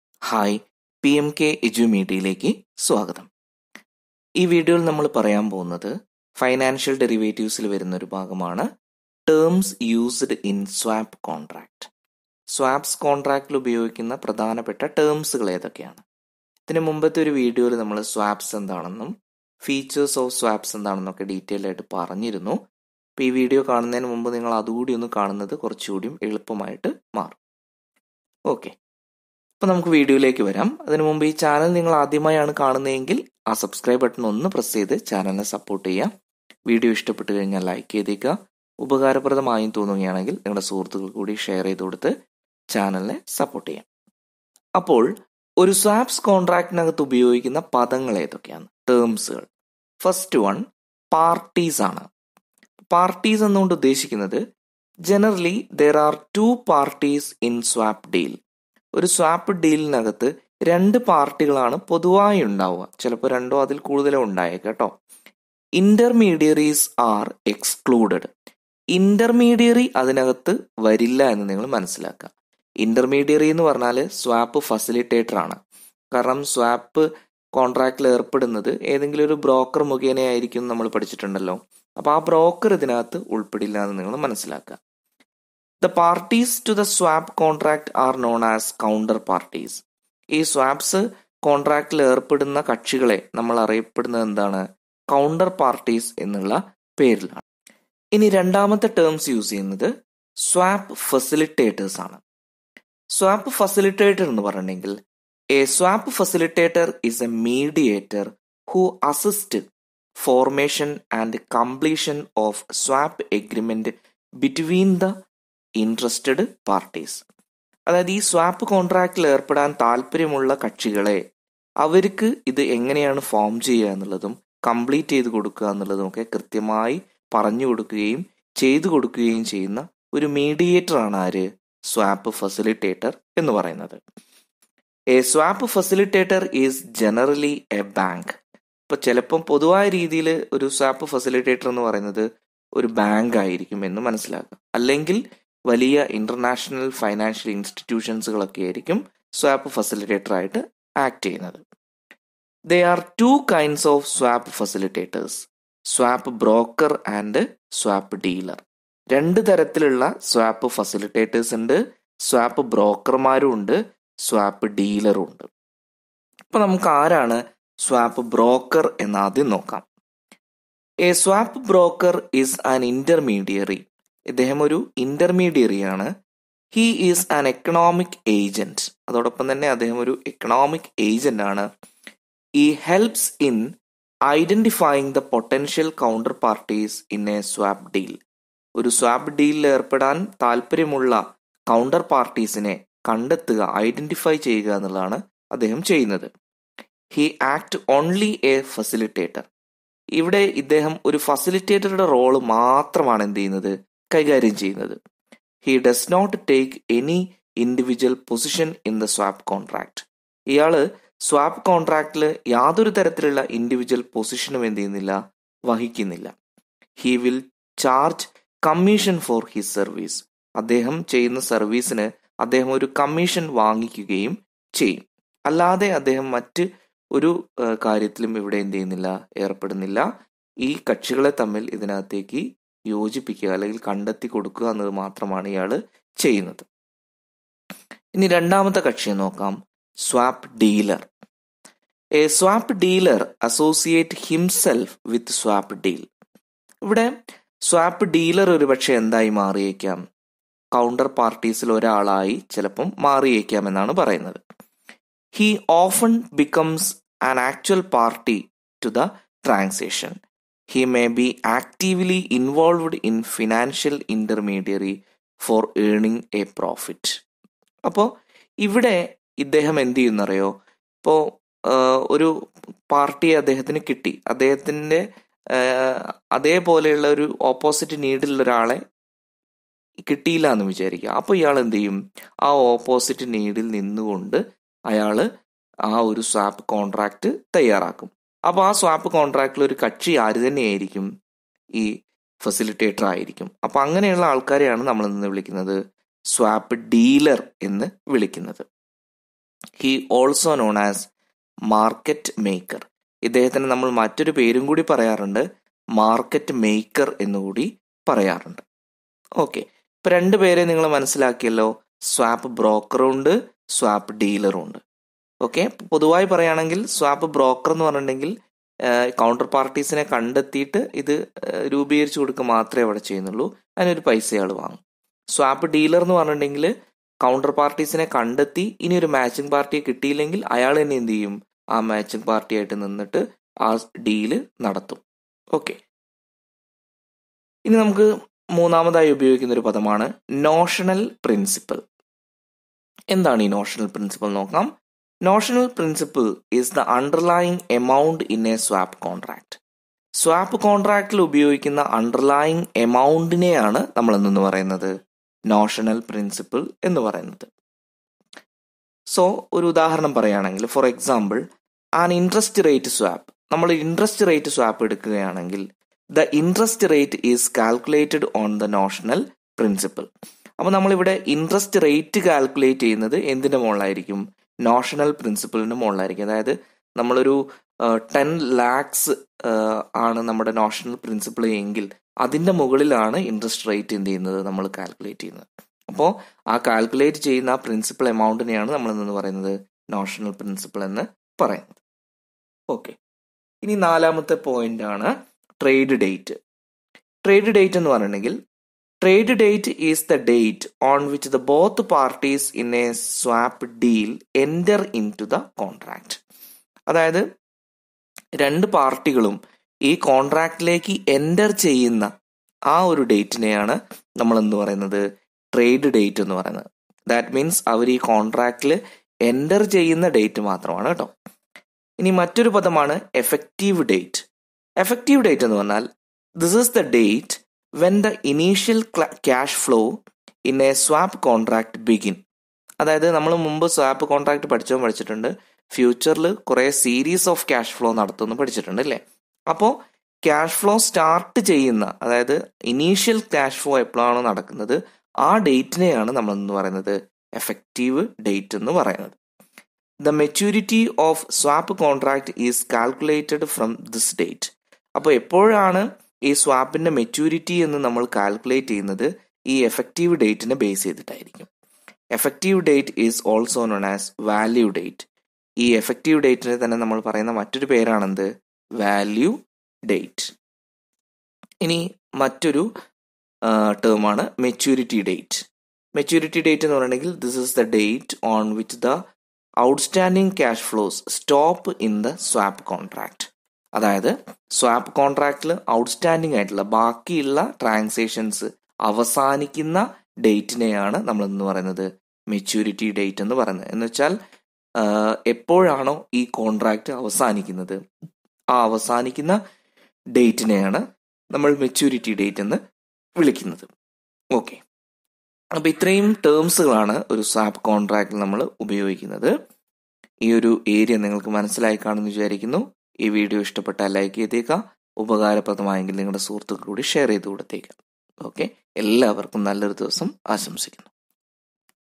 Terms used in Swap Contracts | Financial Derivative | Malayalam
Content : This video explains the terms used in swap contracts.